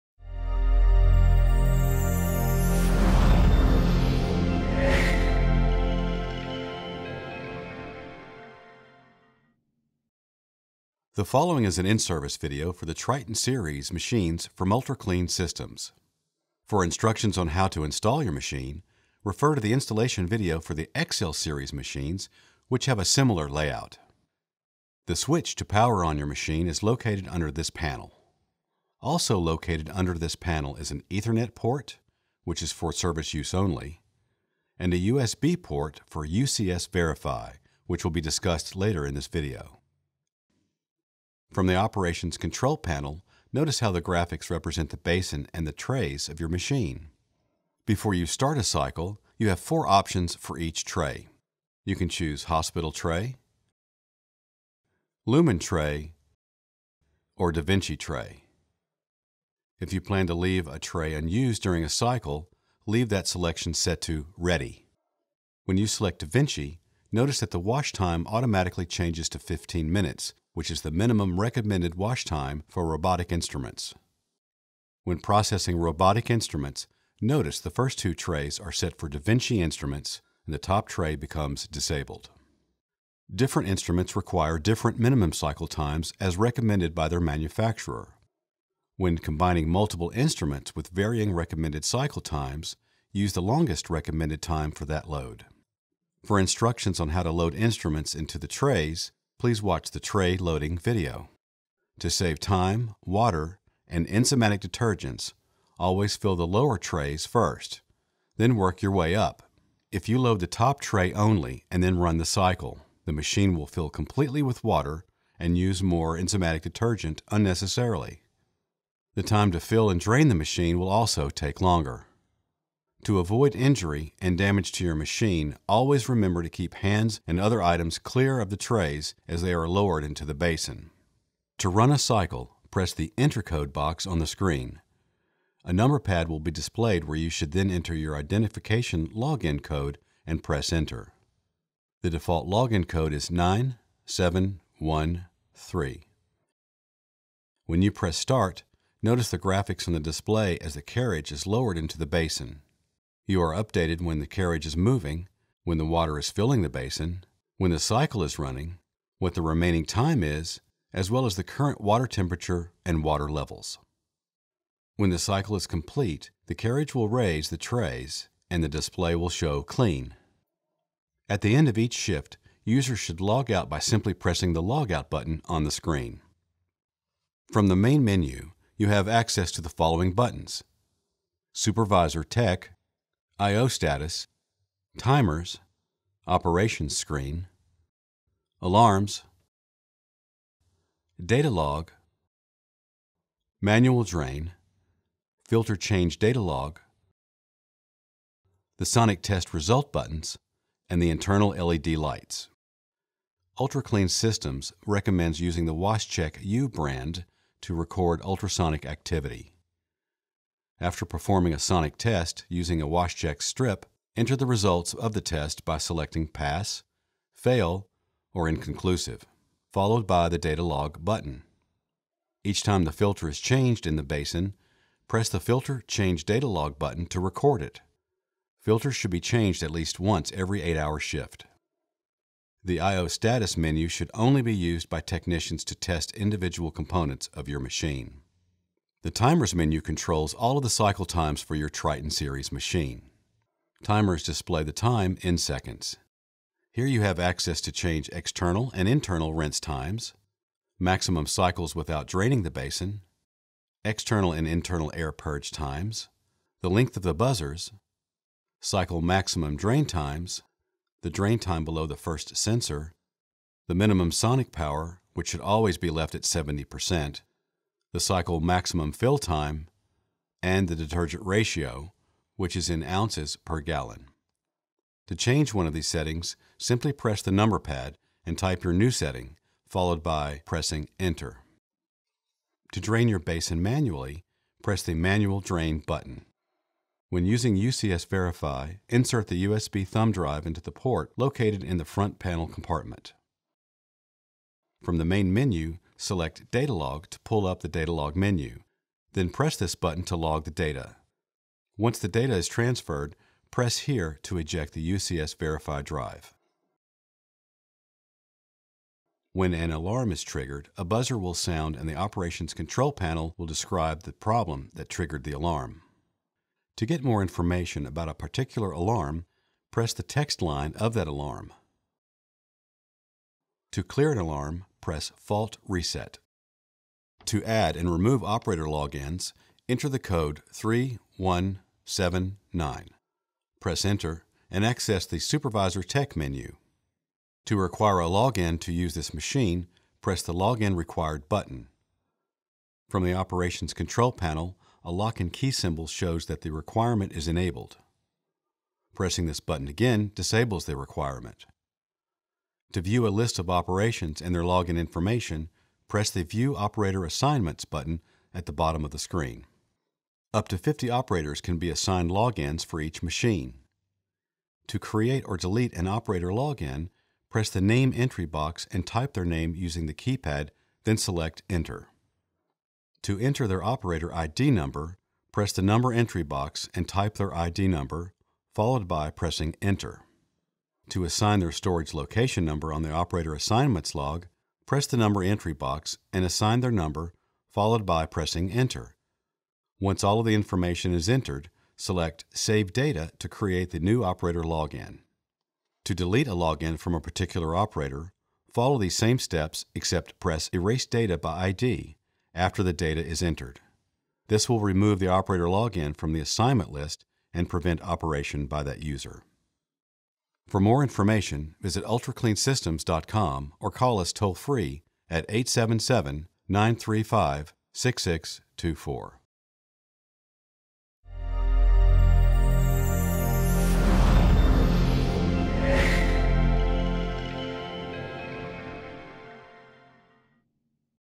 Triton Series In-Service Video
Designed to keep up with the hectic schedule of today’s Central Sterile Departments, UCS proudly offers the TRITON Series Ultrasonic Surgical Instrument Cleaning Systems, which are capable of cleaning even the longest robotic instruments.

These workhorse Ultrasonic Cleaning Systems provides higher throughput with its ability to clean more instruments in less time, which helps your department run more efficiently!

TRT Technology promotes better cavitation than standard ultrasonics with a cycle time of 13 minutes (15 Minute Dedicated da Vinci® Cycle)! This exclusive technology provides superior cavitation allowing the user to clean more instruments in a shorter amount of time.

The Triton is the ultimate in surgical instrument cleaning with its water sterilization feature. The incoming and recirculated water is treated with ultraviolet energy to deactivate bacteria and viruses.

A pneumatic air cylinder safely control raising and lowering of the work tray elevator. Eliminates the need for operators to reach into the tank of contaminated solution and reduces back strain.

This cleaning system is unparalleled in the industry!

Triton 36 Automatic Single Basin

13 minute Ultrasonic Cleaning Cycle
15 Minute Dedicated da Vinci® Cycle
Industry exclusive TRT (Titanium Rod Transducer)
Ultraviolet Water Deactivator
Automatic Load Tray Elevator
Slide and Lock Baskets: Simple and efficient tray loading
36 dedicated ports for cannulated instruments with 3 baskets
20 da Vinci® Robotic instruments per cycle
40 pounds total weight capacity - Keep sets together and productivity up
Dual purpose: Can be used with hospital baskets
Easy-to-use large graphical touch-screen with programmable timer
Fully automatic degas, wash, rinse and air purge completely eliminates waiting time for degassing
Tap, RO or DI water compatible
208-Volt wall receptacle
24”W x 54”D x 63”H (lid open)
30 Gallon Basin Capacity

For large processing demands, our Triton 72 rises to the top with productivity.

The Triton 72 can process 4 complete sets of Xi series robotics in 13 minutes. This complete system is user friendly with a 10 inch touchscreen and includes automatic lifts with slide & lock baskets. The Triton Series ultrasonic cleaning systems have more cavitation cleaning power than any machine available

Triton 72 Automatic Double Basin

13 minute Ultrasonic Cleaning Cycle
15 Minute Dedicated da Vinci® Cycle
UV Water Deactivator
72 dedicated ports for cannulated instruments with 6 baskets
40 Robotic instruments per cycle
80 pounds total weight capacity
Easy-to-use 10 inch touch-screen with programmable timer
Accepts hospital baskets
208-Volt wall receptacle
34”W x 54”D x 63”H (lid open)
30 Gallon Basin Capacity (x2 Basins)